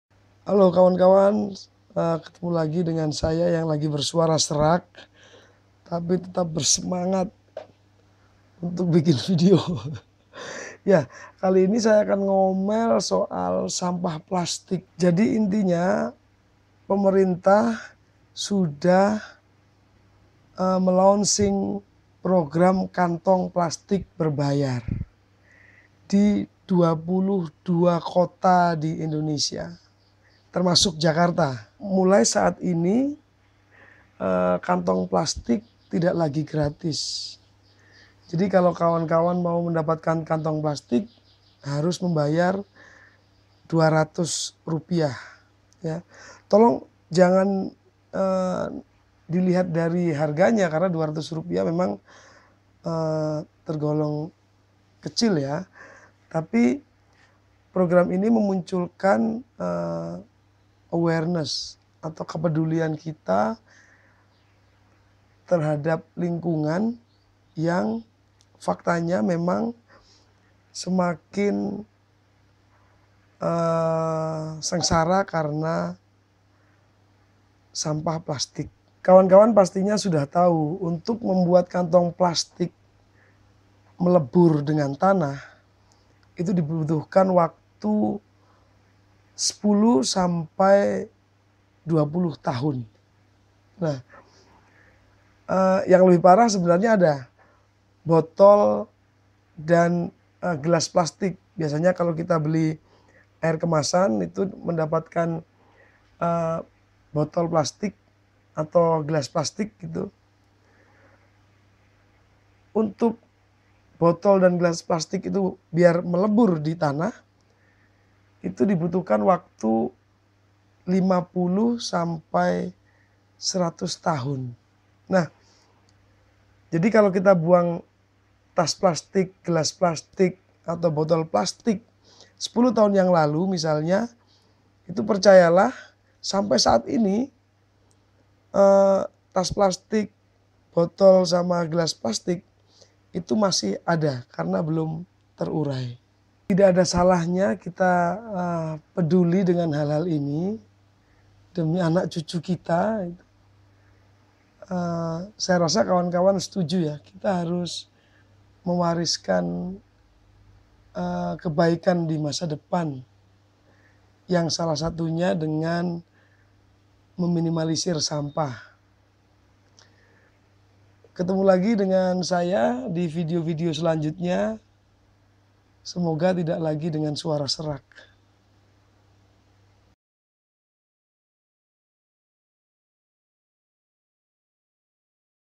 PLASTIK BERBAYAR
Sejak 21 Februari 2016, tas plastik tidak lagi gratis alias berbayar. Program ini diluncurkan pemerintah untuk memunculkan kepedulian publik pada lingkungan, dengan menumbuhkan rasa bijak dalam penggunaan tas plastik (akrab juga disebut tas kresek-red). Sejak diluncurkan dan berlaku di 22 kota di Indonesia, publik harus membayar Rp200,- untuk satu tas plastik.